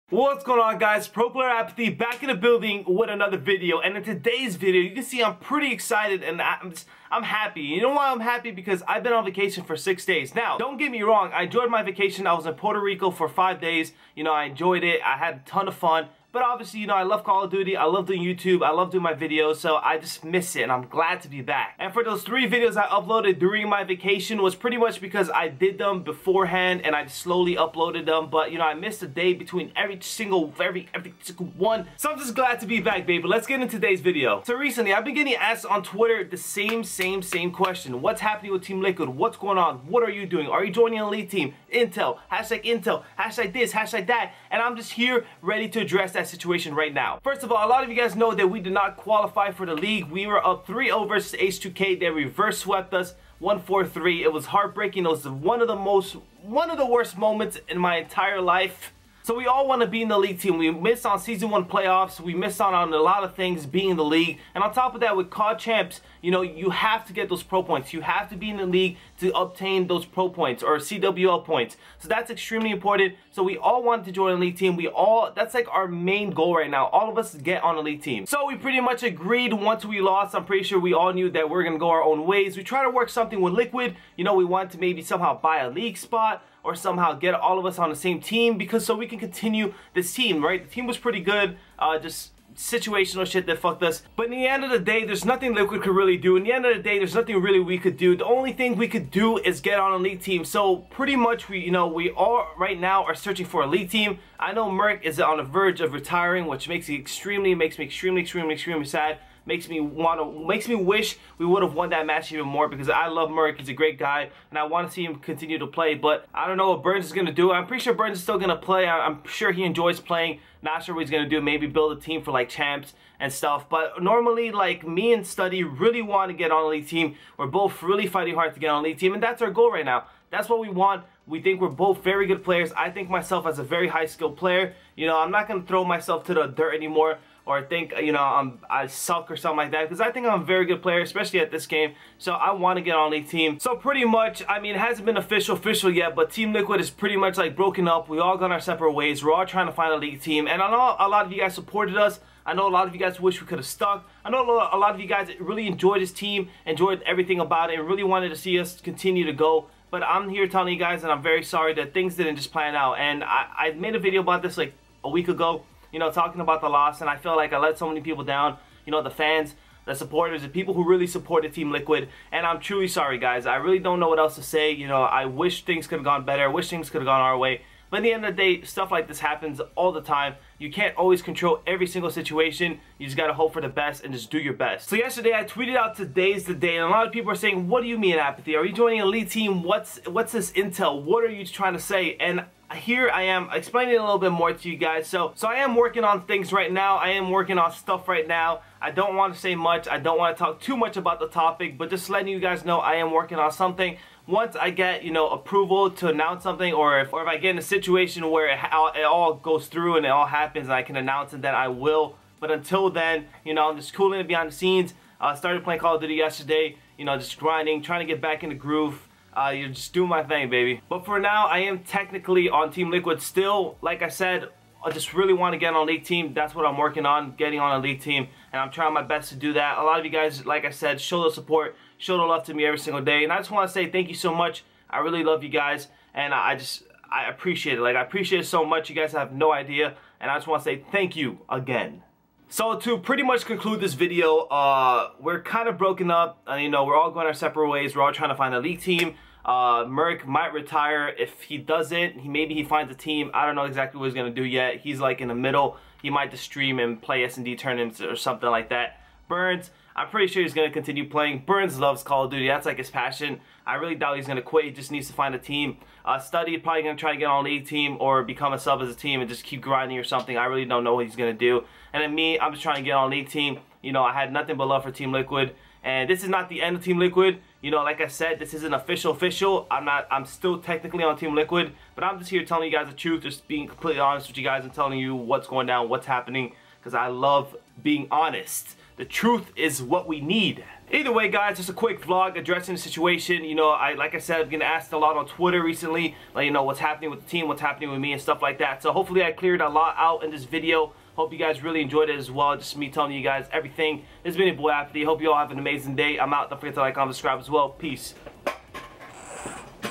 WHATS HAPPENING W/ TEAM LIQUID?!
Update video talking about my team & future plans